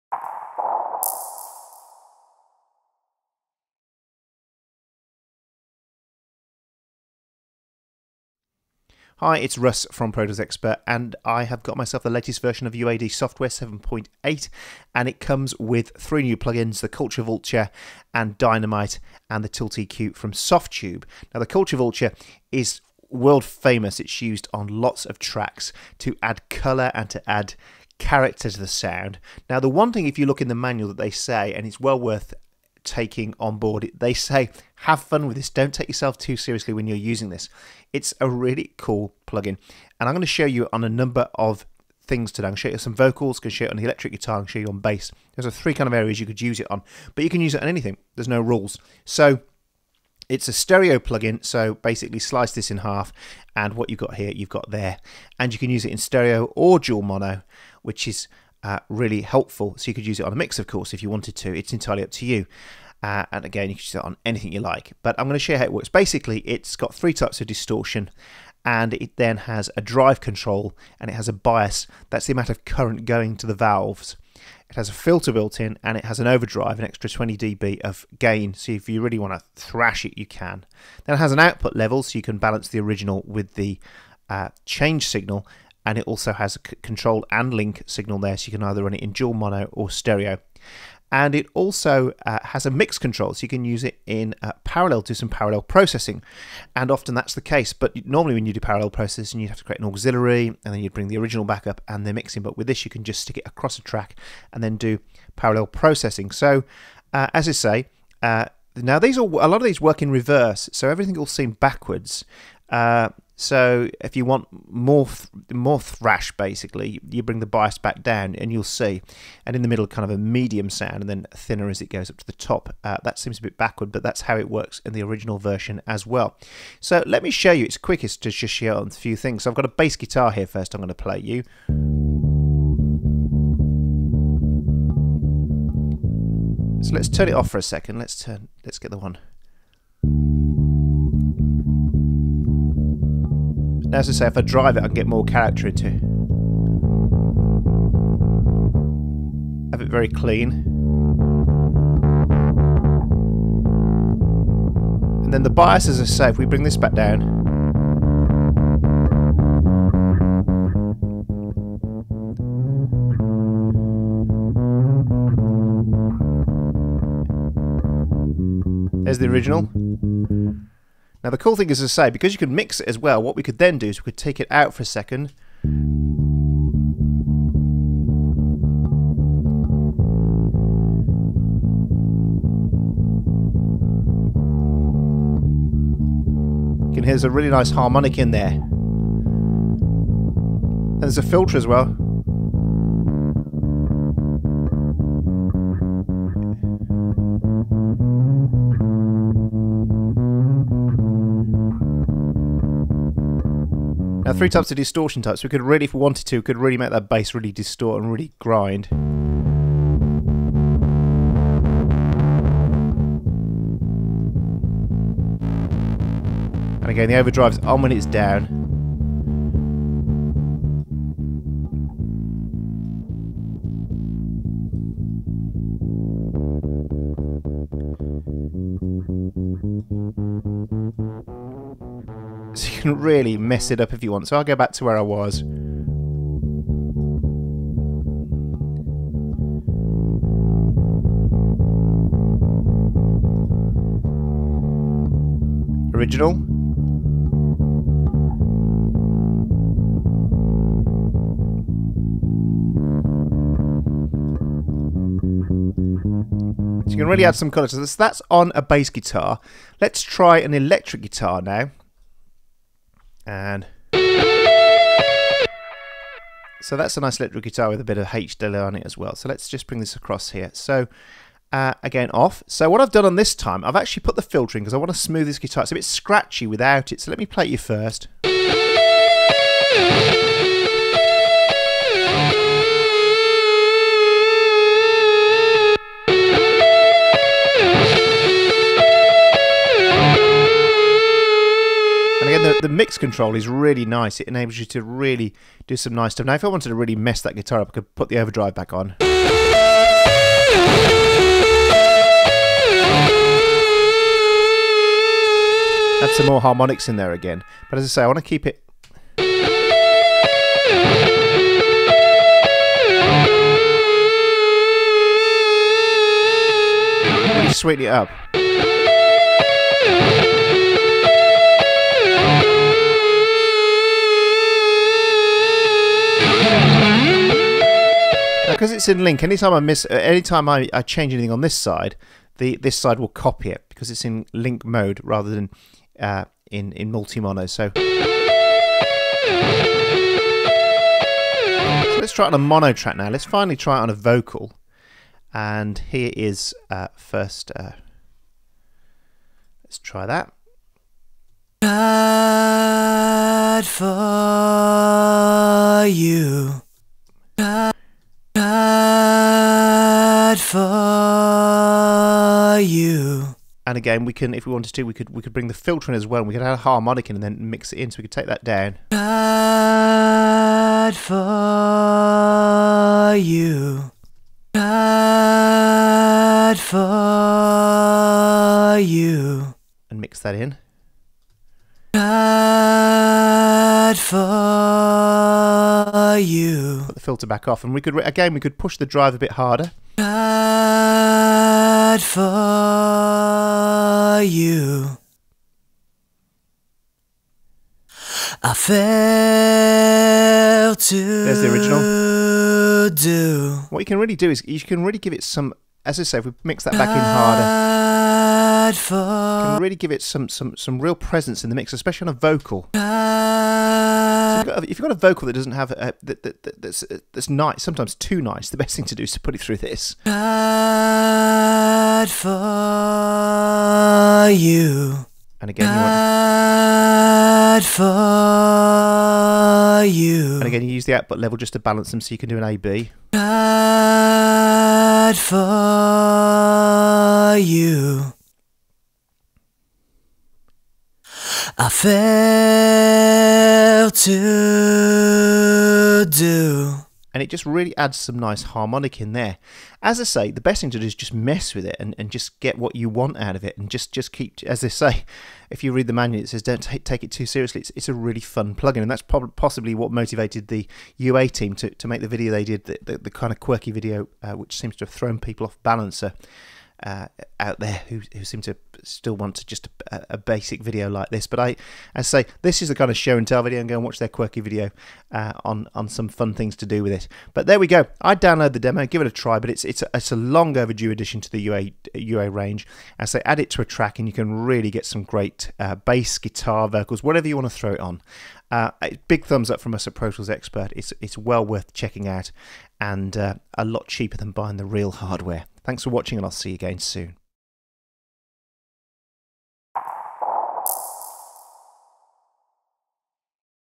UAD Thermionic Culture Vulture Review
Russ takes and look and listen to the latest addition to the Universal Audio UAD stable the British favourite Thermionic Culture Vulture.

He gives it a run on bass, guitar and vocals and shows the fun that can be had with this unique audio plug-in.